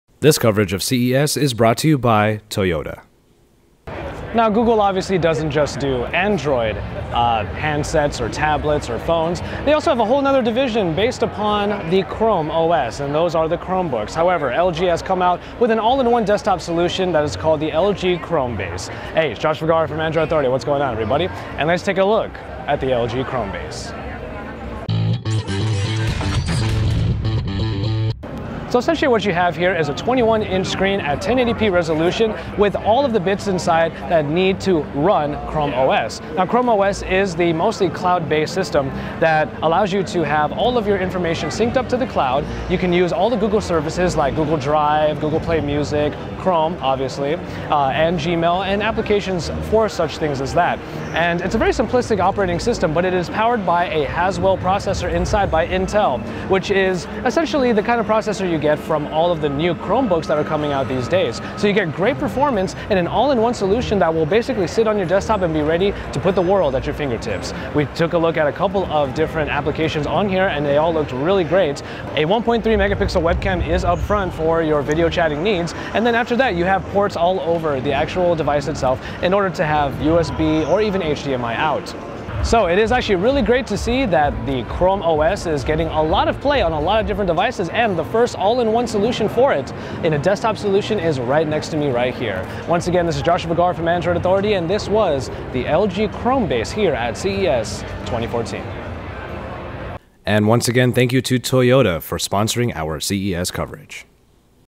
LG Chromebase Hands On! [CES 2014]
The Chormebook line makes it onto the desktop, courtesy of LG. Josh checks out the LG Chromebase.